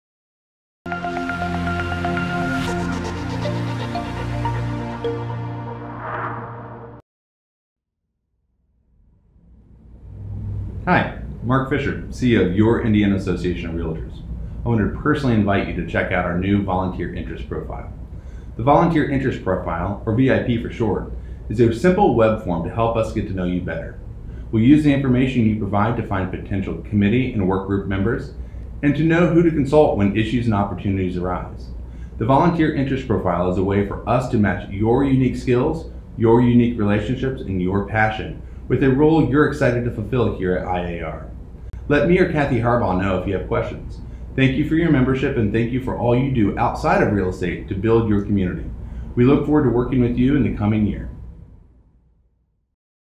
IAR Volunteer Interest Profile
Allow us to introduce the Volunteer Interest Profile.

It’s a simple web form to help us get to know you better. We will use this information to find potential committee and workgroup members, and to know who to consult when issues and opportunities arise.

This form is not an application; it is a way to build our bench. We may be in touch soon, but more than likely it will be later in the coming year when we start to see the impact of term limits and the unveiling of new programs.